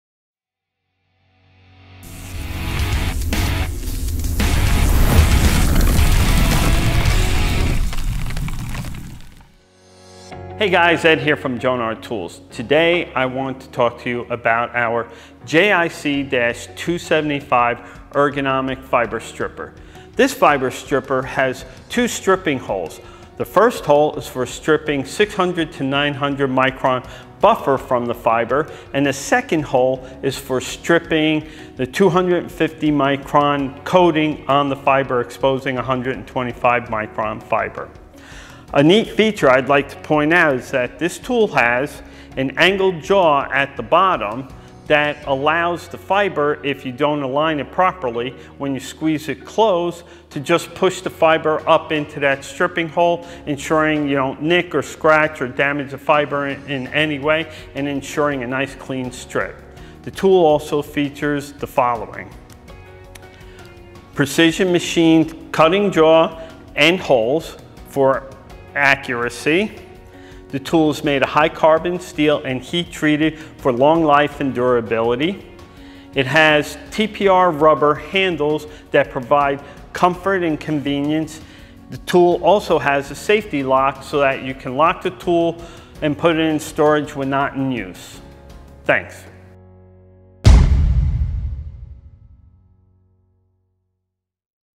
Jonard Tools Fiber Optic Stripper, Two Hole (JIC-275) Product Video
The Jonard Tools JIC-275 Two Hole Fiber Optic Stripper performs the most common fiber stripping functions. This fiber optic stripper features the following:

Strips the 600-900 micron buffer coating, and 250 micron coating to expose the 125 micron glass fiber
The first hole then strips the micro buffer coating to the micron cable
The second hole strips the micron cable to the glass fiber without nicks or scratches
Angled jaw guides fiber into the 2nd hole to prevent accidental cutting of the fiber
Safety lock allows the tool to be easily stored when not in use
Tool is made of strong high carbon steel and the handle is made of TPR (thermoplastic rubber) for high durability and greater comfort